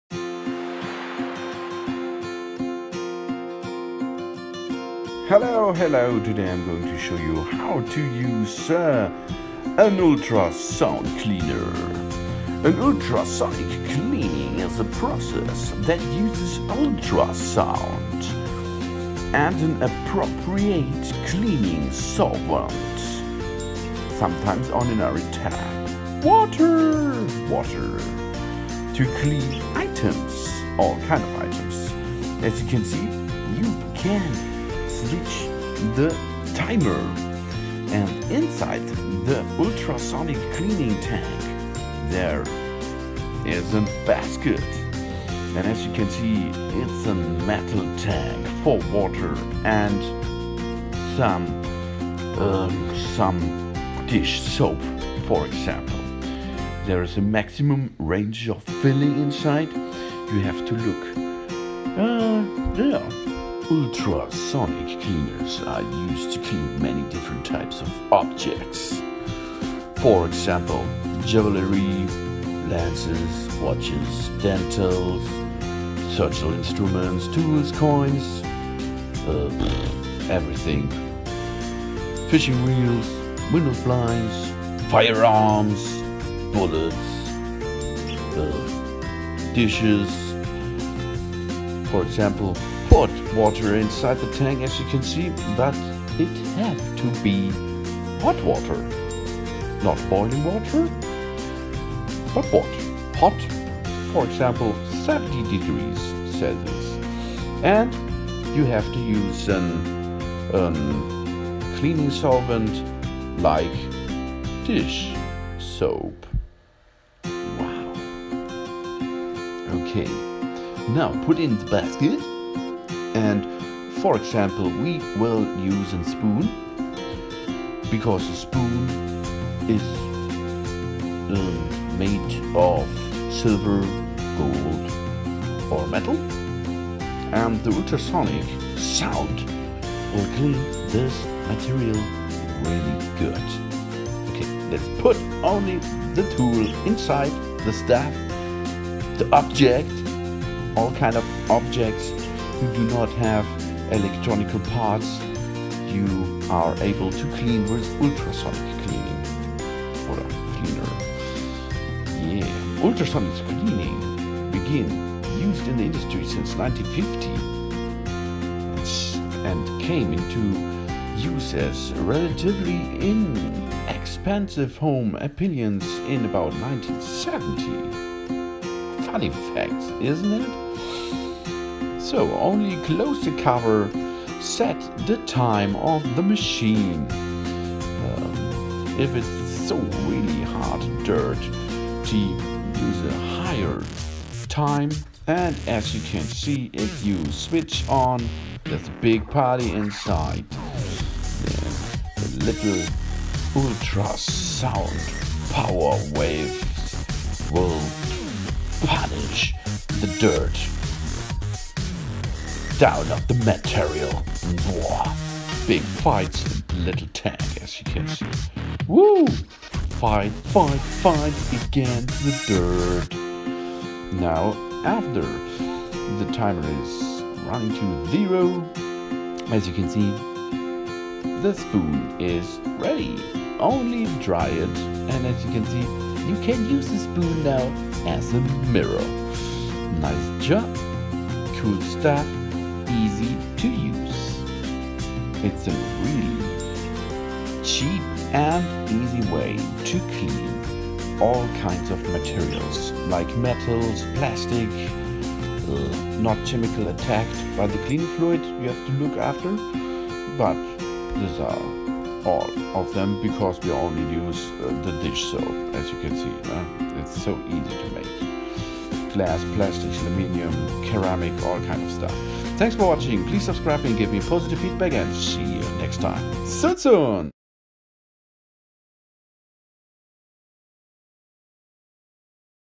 How to use an ultrasound cleaner ultrasonic cleaner hands-on Video DIY
Ultrasonic cleaning is a process that uses ultrasound (usually from 20–400 kHz) and an appropriate cleaning solvent (sometimes ordinary tap water) to clean items. The ultrasound can be used with just water, but use of a solvent appropriate for the item to be cleaned and the type of soiling present enhances the effect. Cleaning normally lasts between three and six minutes, but can also exceed 20 minutes, depending on the object to be cleaned.

Ultrasonic cleaners are used to clean many different types of objects, including jewellery, lenses and other optical parts, watches, dental and surgical instruments, tools, coins, fountain pens, golf clubs, fishing reels, window blinds, firearms, car fuel injectors, musical instruments, industrial parts and electronic equipment. They are used in many jewellery workshops, watchmakers' establishments, and electronic repair workshops.

(Source:Wikipedia)